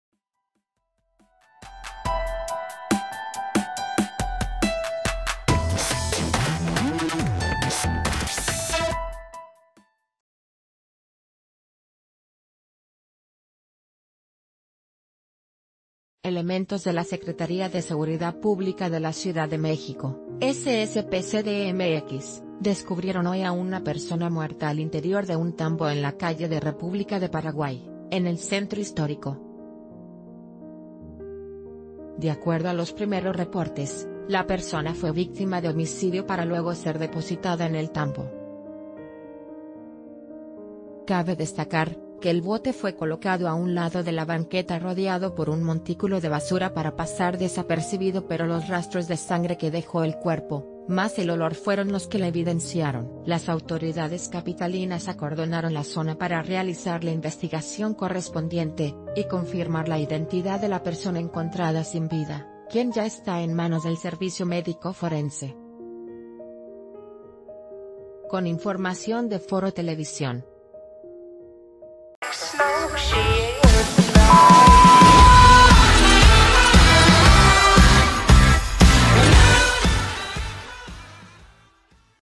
Descubren a persona muerta dentro de un tambo en calles del Centro Histórico
Elementos de la Secretaría de Seguridad Pública de la Ciudad de México (SSPCDMX) descubrieron hoy a una persona muerta al interior de un tambo en la calle de República de Paraguay, en el Centro Histórico. De acuerdo a los primeros reportes, la persona fue víctima de homicidio para luego ser deposita...